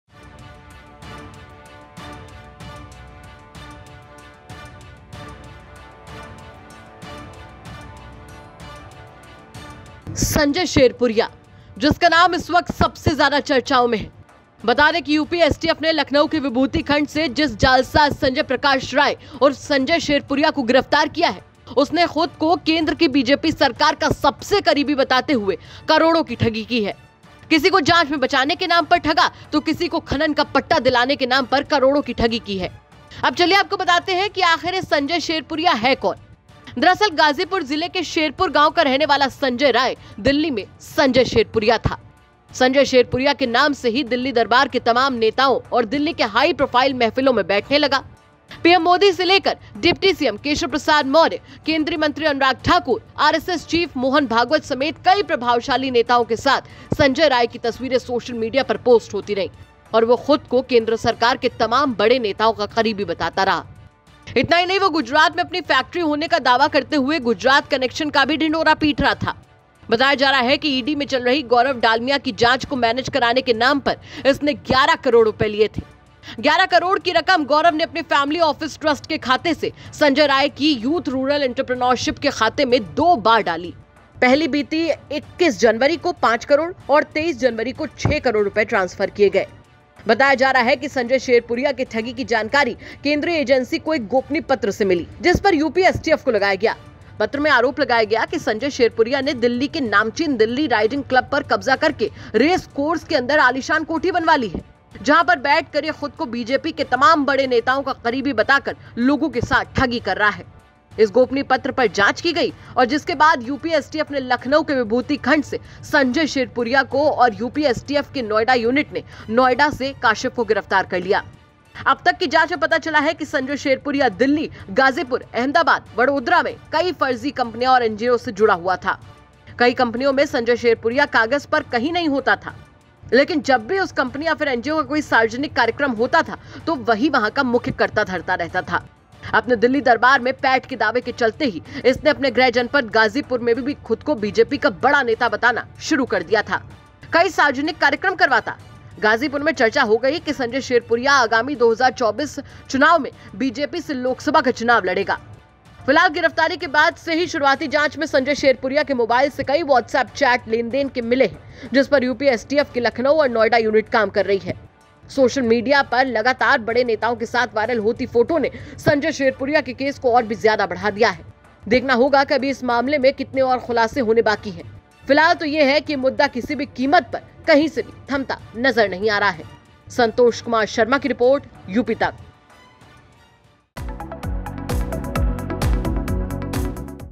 Sanjay Sherpuriya : संजय शेरपुरिया की पूरी कहानी जान लीजिए..उड़ जाएंगे होश!
संजय शेरपुरिया..जिसका नाम इस वक्त सबसे ज्यादा चर्चाओं में है..बता दें कि यूपी एसटीएफ ने लखनऊ के विभूति खंड से जिस जालसाज संजय प्रकाश राय उर्फ संजय शेरपुरिया को गिरफ्तार किया है उसमें खुद को केंद्र की बीजेपी सरकार का सबसे करीबी बताते हुए करोड़ों की ठगी की है। किसी को जांच में बचाने के नाम पर ठगा तो किसी को खनन का पट्टा दिलाने के नाम पर करोड़ों की ठगी की है..